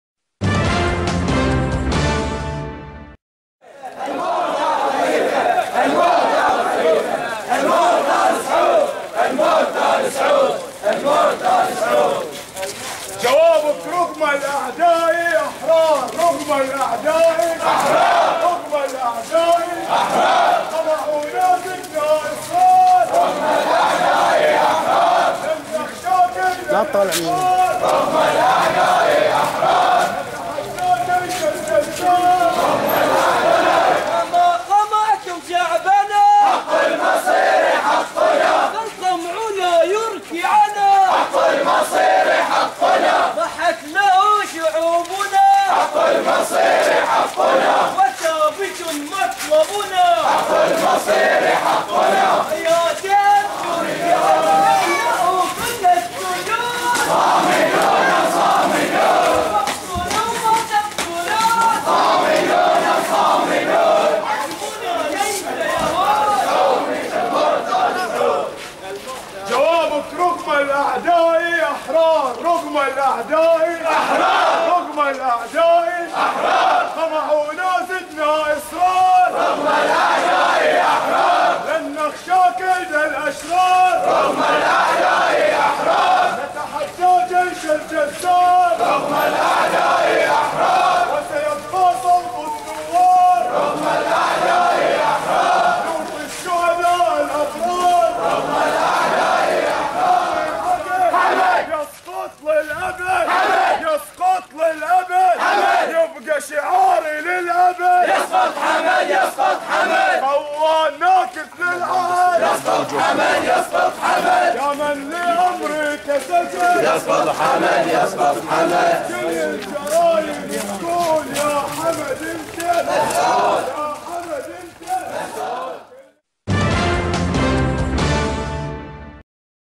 المئات في مسيرة "أرواحنا فداء لقادة الإباء" في منطقة الحجر 3/1/2014 Bahrain
شاركت حشود غفيرة مساء اليوم الجمعة (3 يناير/كانون الثاني 2014) في مسيرة خرجت بمنطقة الحجر غرب العاصمة المنامة تضامنا مع القيادات السياسية المعتقلة، وتأكيداً على حقهم الثابت في الحرية، المشاركون حملوا أعلام البحرين وصور الشهداء والقيادات السياسية، وردد المتظاهرين شعارات مناهضة للنظام الخليفي، وشعارات تنادي بإسقاط حاكم البحرين حمد بن عيسى آل خليفة.